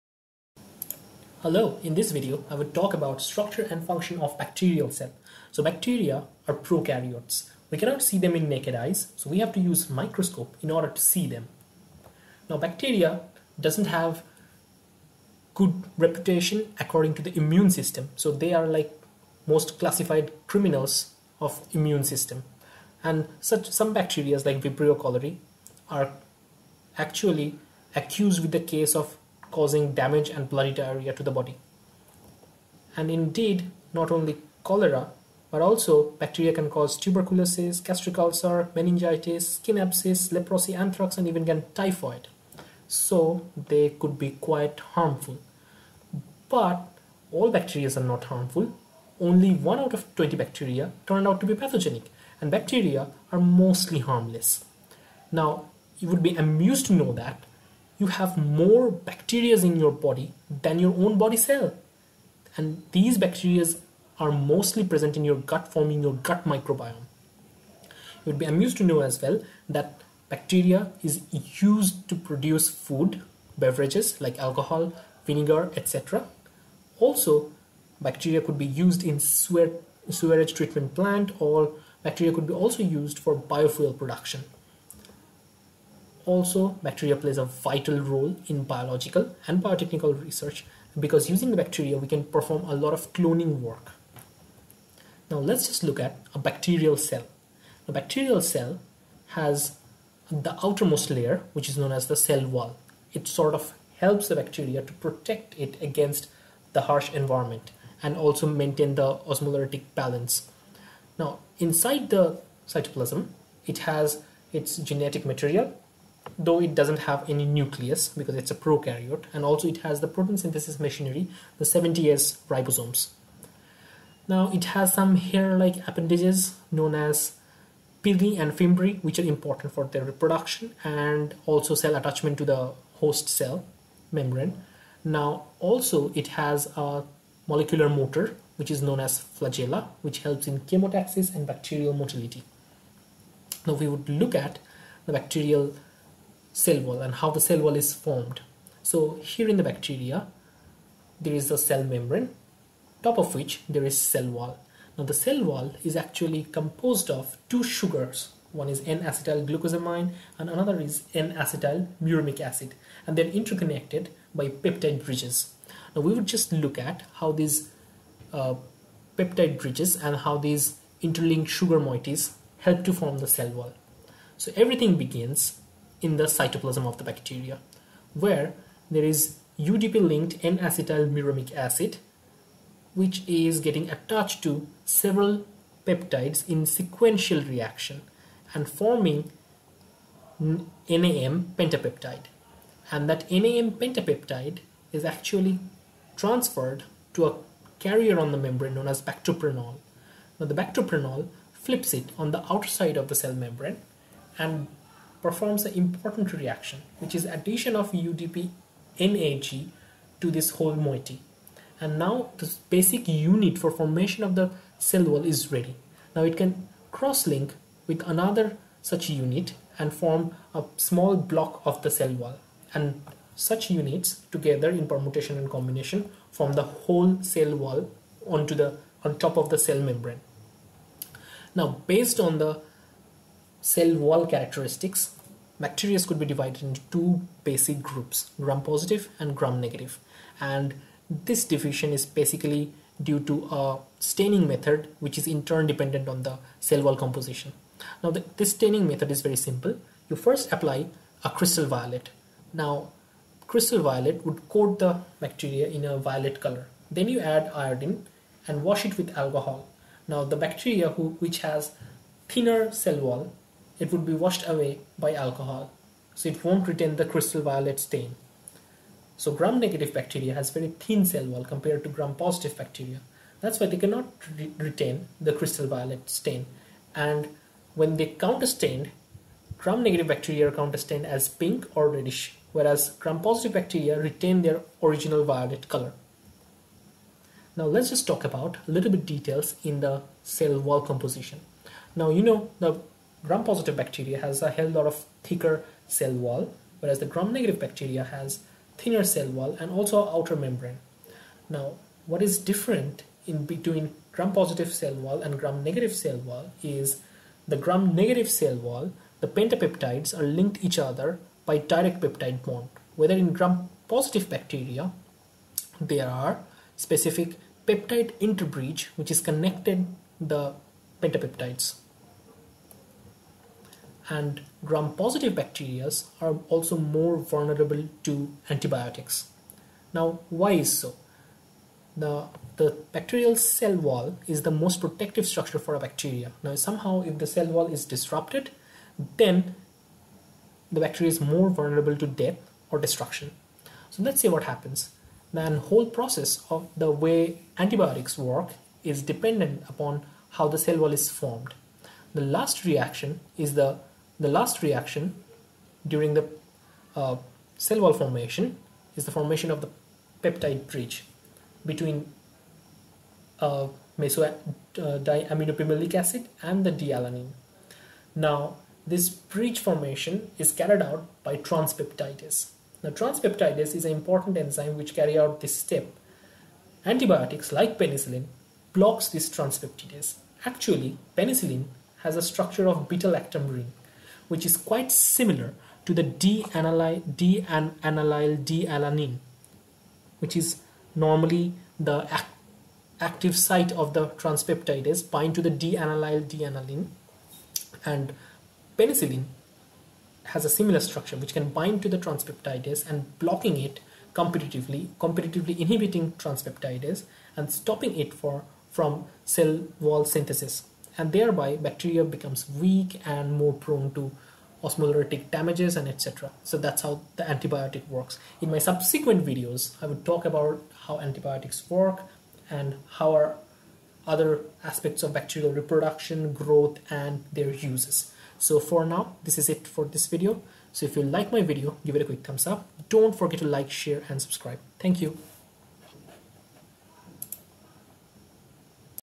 Bacteria : structure ,function and utility overview
This video describes briefly about structure and function of several parts of bacterial cells in the bird's eye view.